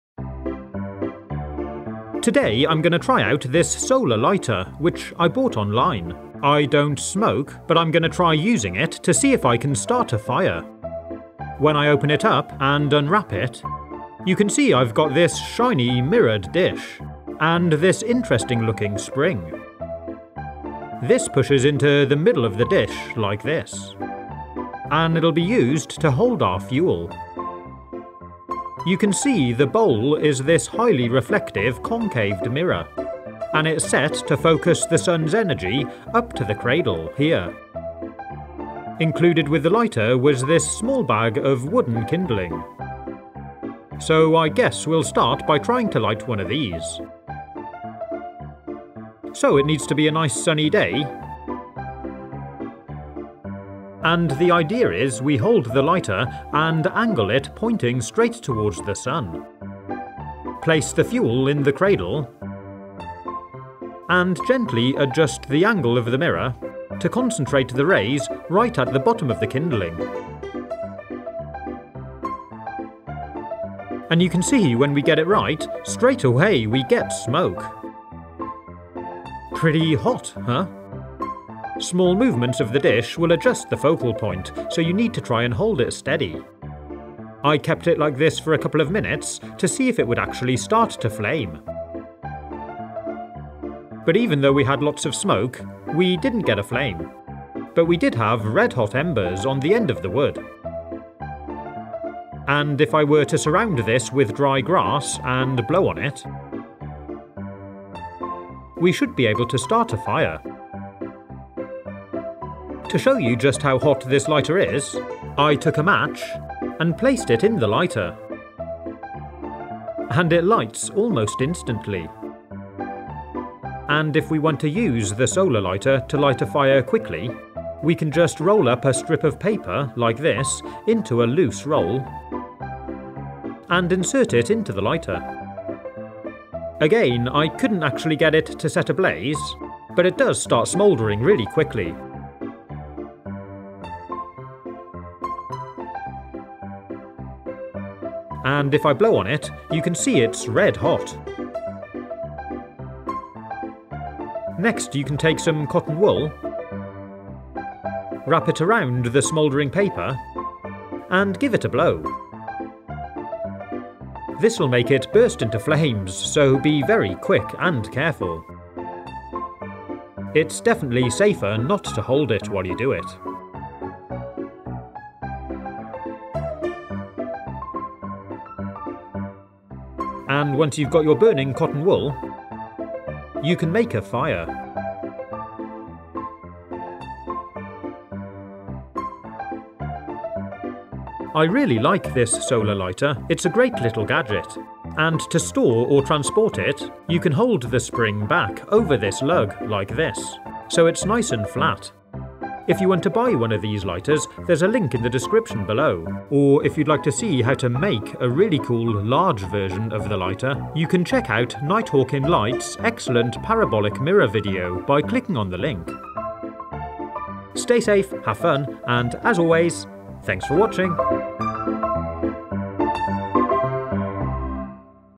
Amazing Solar Lighter
Testing out this sun powered lighter. Will it light a fire? This highly reflective concave parabolic mirror focuses the suns energy into an extremely hot spot.